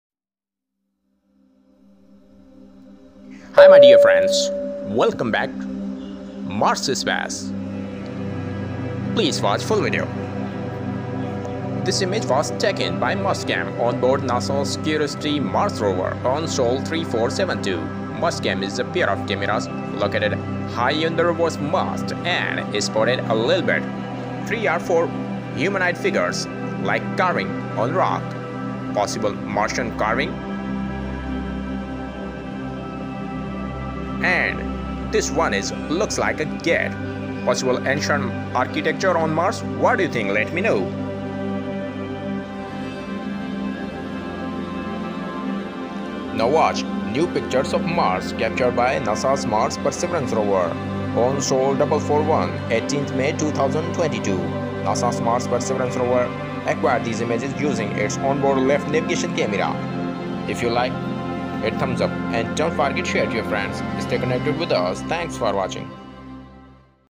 NASA's Mars Curiosity Rover Capture This New Image on Red Planet Surface on Sol 3472 | Perseverance
in this video we have shown some humanoid figures carving on rock spotted by Curiosity Rover with latest images of mars from NASA's Perseverance rover of its mission.

Credit: NASA.GOV
NASA/JPL-Caltech